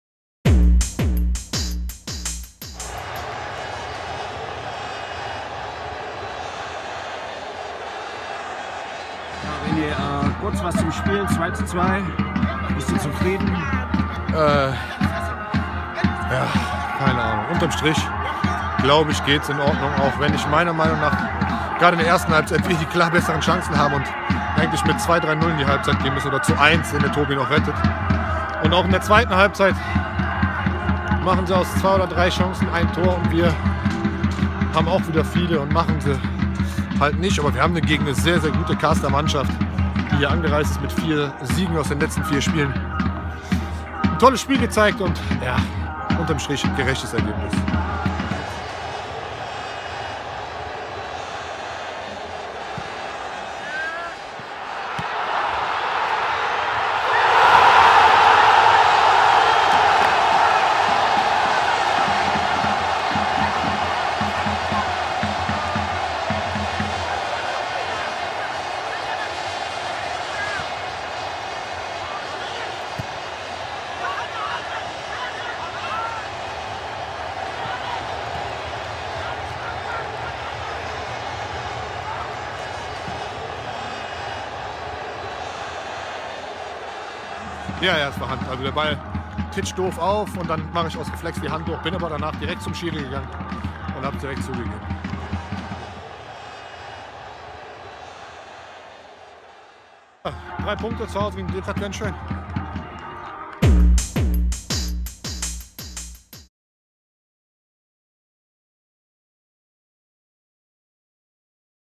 René Schnitzler nach dem 2:2 gegen Kaarst
René Schnitzler im Interview nach dem 2:2-Unentschieden gegen Kaarst